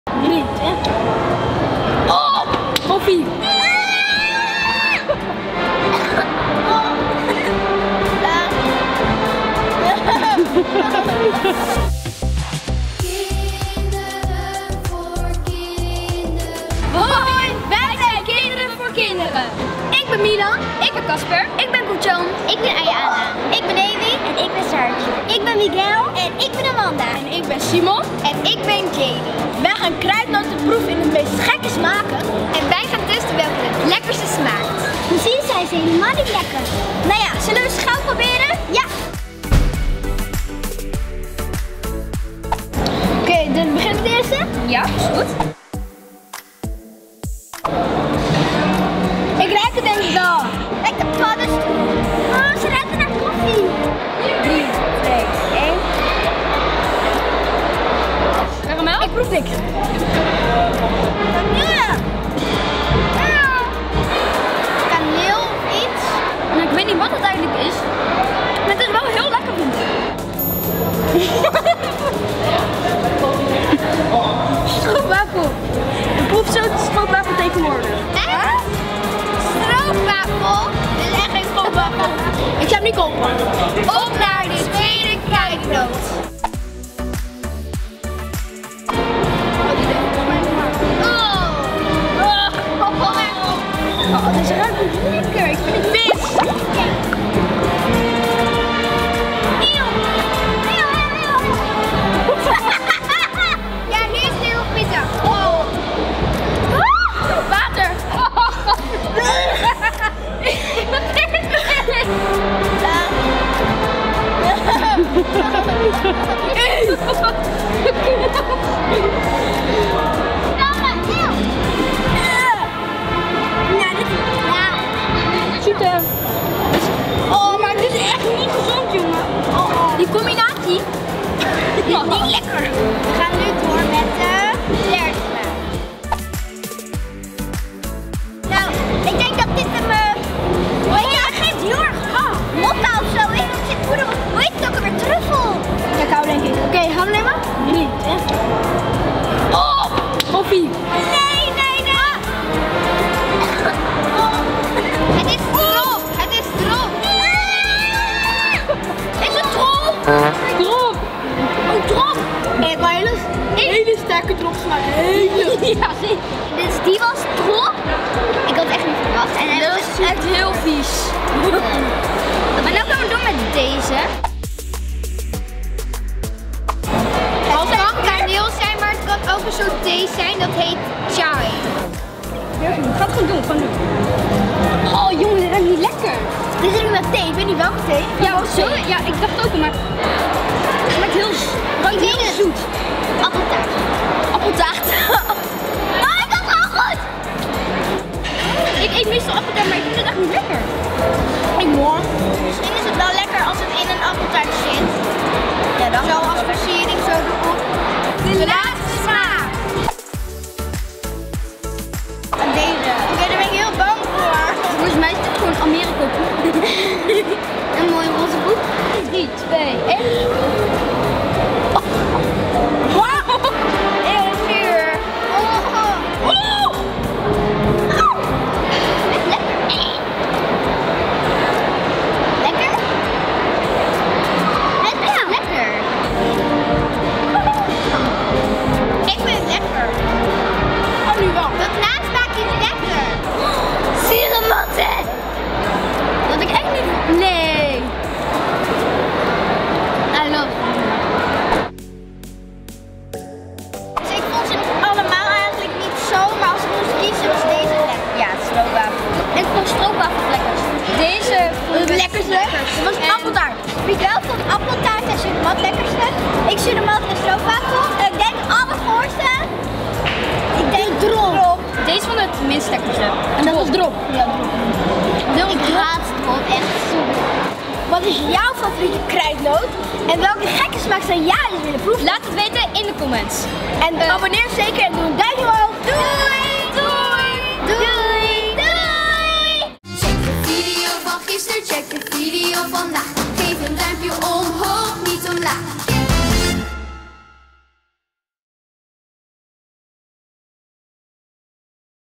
GEKKE KRUIDNOTEN PROEVEN! - Kinderen voor Kinderen
Kinderen voor Kinderen proeft de meest gekke kruidnoten! Van dropsmaak tot zure matten-smaak! Zou jij het aandurven?